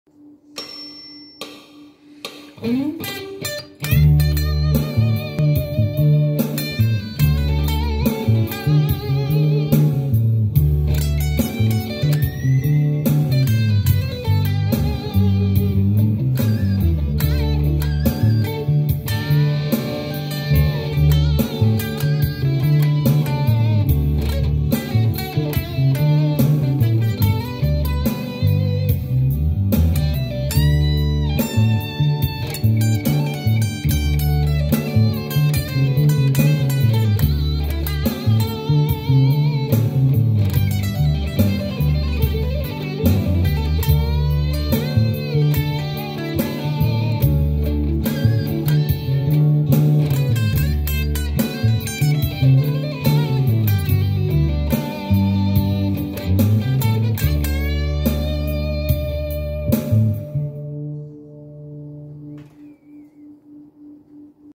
Key of 1 - Part 4
Last jam on chords D Dmaj7 D7 G
It would be great to see and hear you jams, to perhaps load up part 2, and share you improvising?
Have great week...